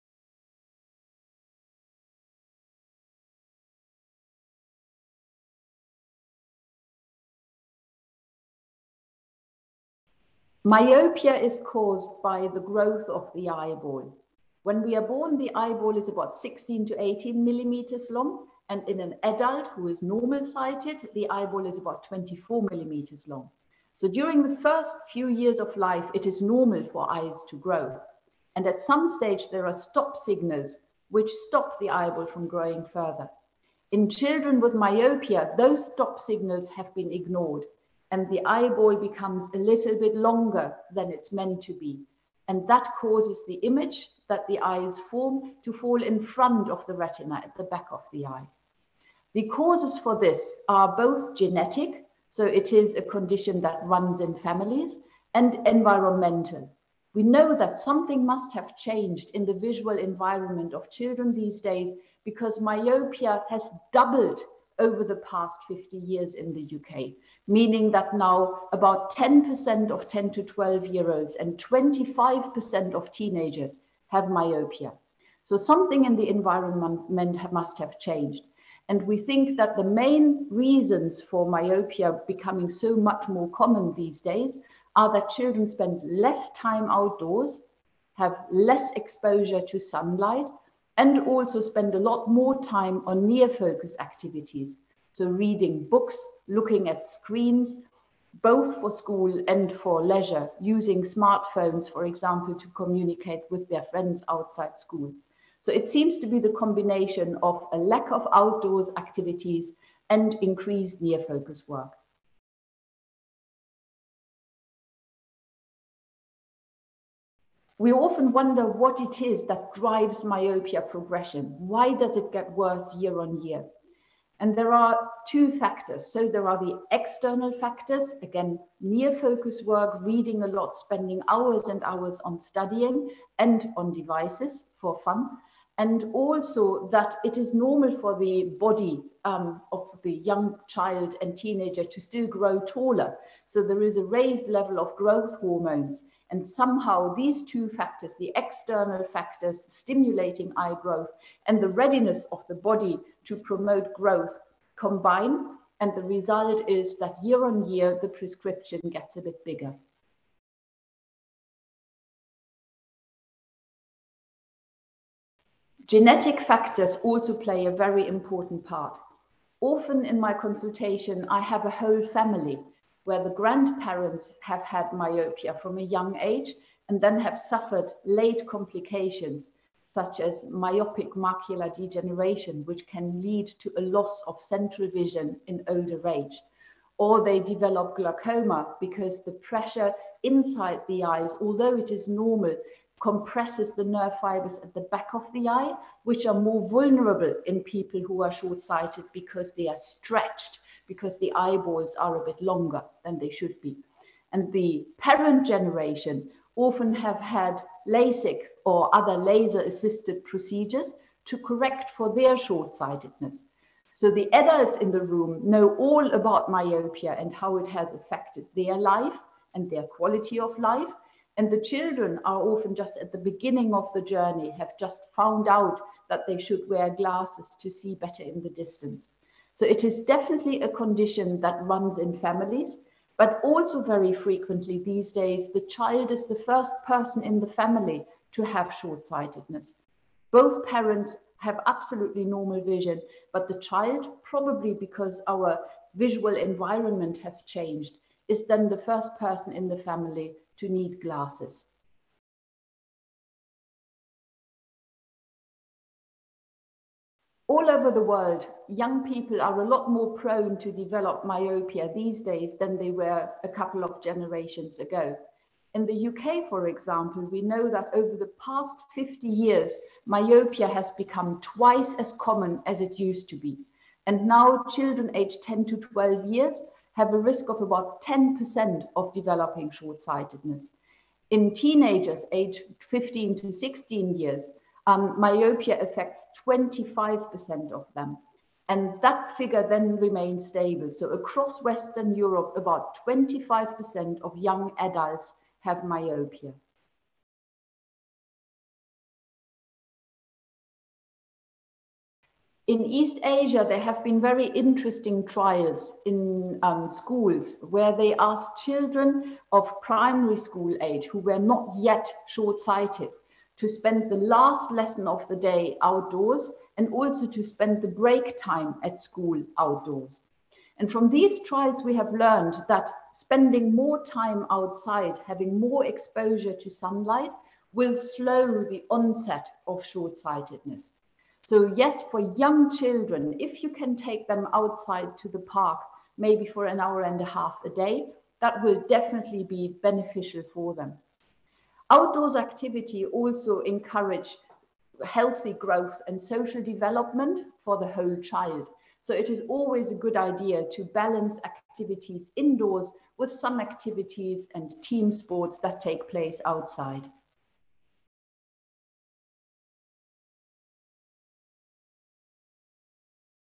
What causes nearsightedness/myopia in children? - Online interview
“We know that something must have changed in the environment of children these days because myopia has doubled over the past 50 years in the UK.”

Esteemed eye clinician based in London, Dr Annegret Dahlmann-Noor answers some of the most common questions about near-sightedness in children in our latest online video. Dr Dahlmann-Noor explains the causes, what aggravates the conditions, possible ways to reduce the risk of developing it, and more.

00:00 What causes nearsightedness/myopia in children?
01:53 What causes it to get worse?
02:41 Do genes have a part to play in developing myopia?
04:13 Are young people more likely to develop myopia?
04:58 Are there ways to reduce the likelihood of causing myopia, such as playing outside?